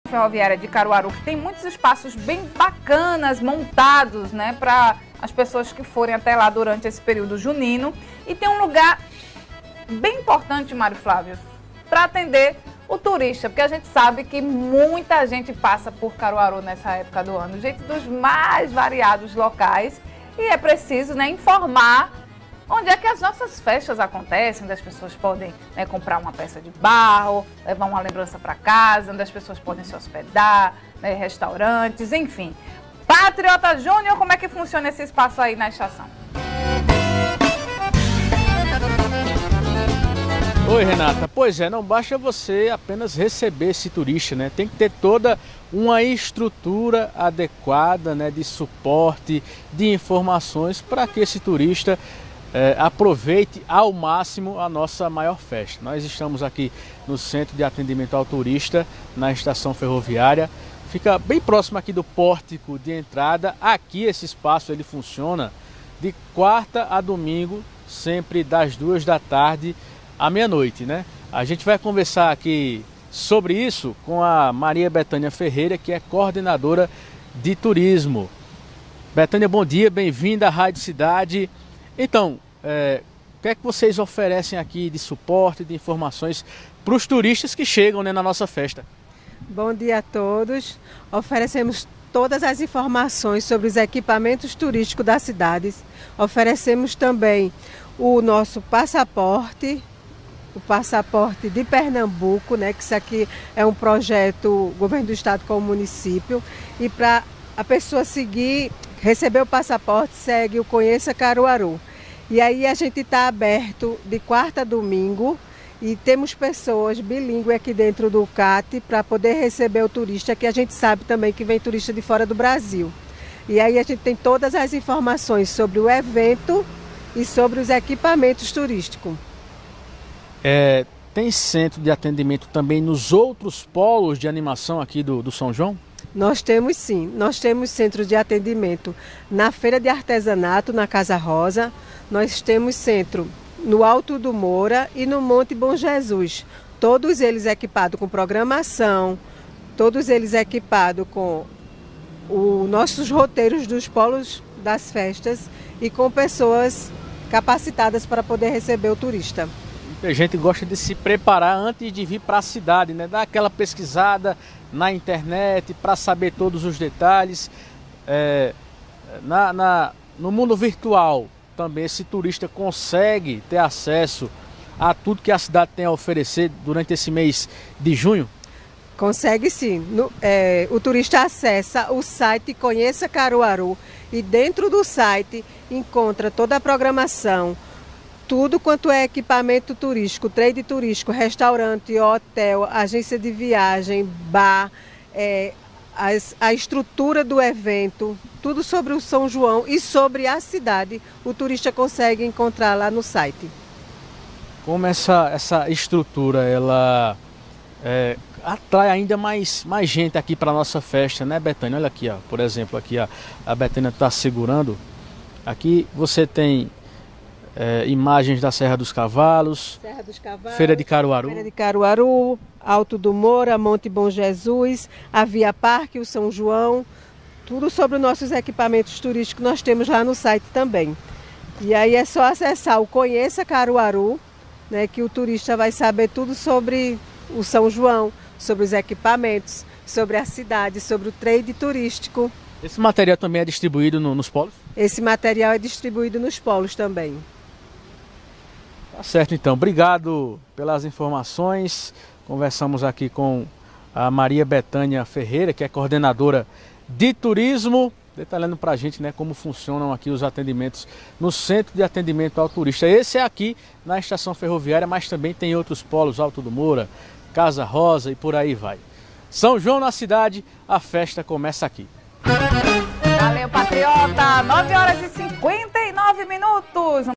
São João na Cidade 21.06.2024 – Confira como funciona o Centro de Atendimento ao Turista no SJ
Durante o São João de Caruaru tem alguns espaços bacanas para as pessoas que vão curtir a festa, um desses lugares é Centro de Atendimento ao Turista (CAT). O repórter Patriota Júnior conversou sobre o assunto com a coordenadora de Turismo, Maria Betânia. Confira.